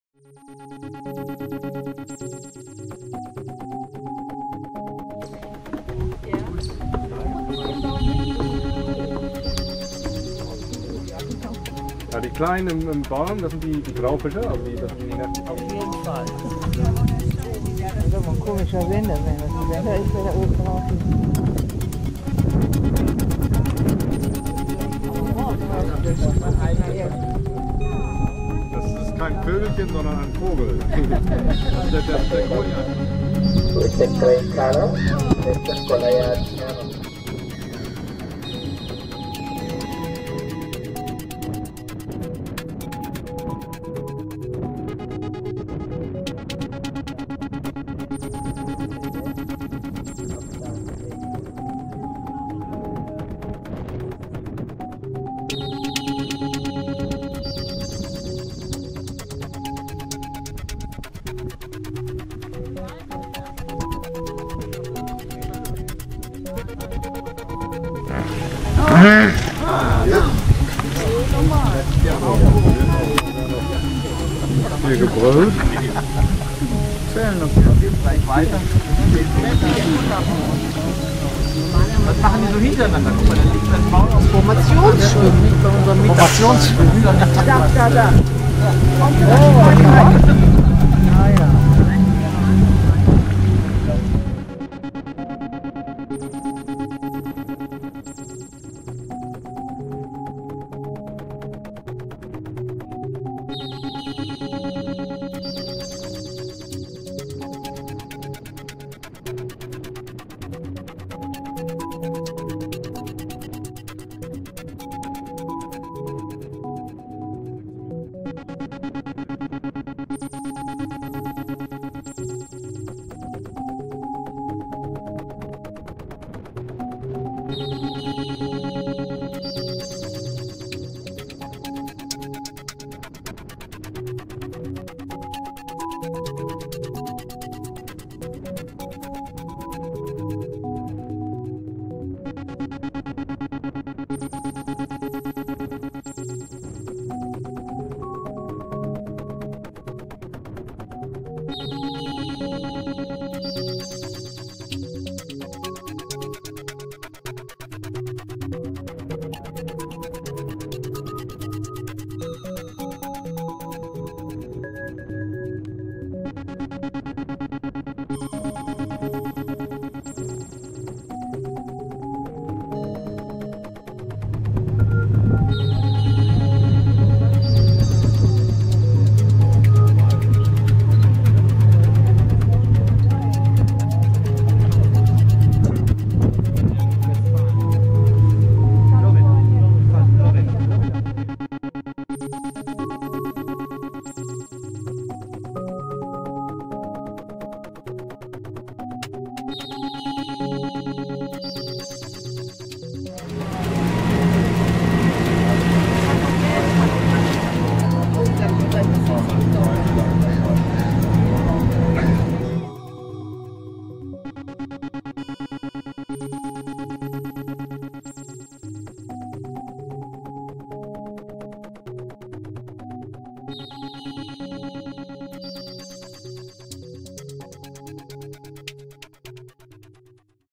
Südafrika 2012: Santa Lucia
Fahrt mit dem Boot in die Lagune des iSimangaliso Wetland Park bei Santa Lucia.